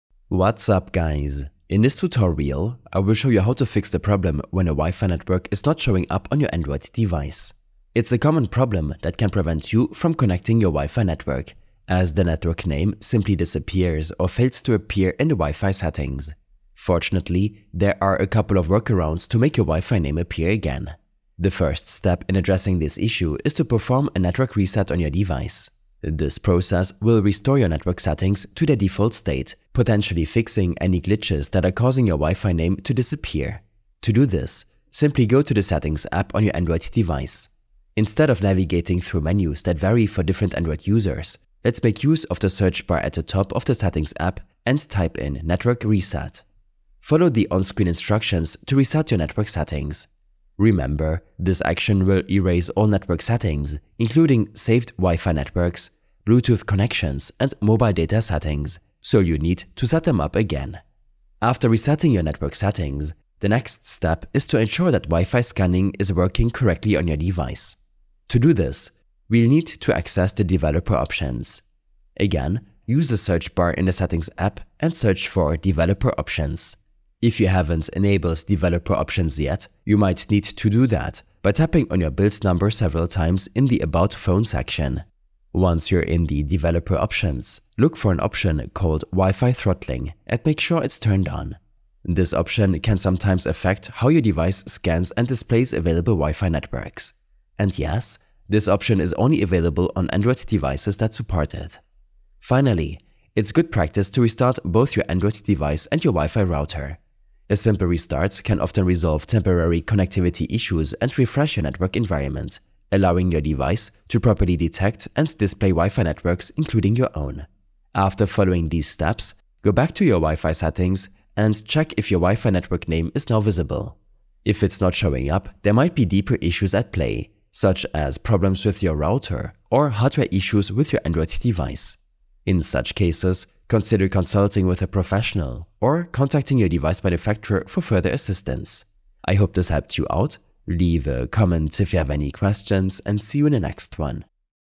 How To Fix WiFi Name Not Showing Up on Android - Not Detecting WiFi
A short tutorial on fixing the problem when a WiFi network is not showing up on your Android device.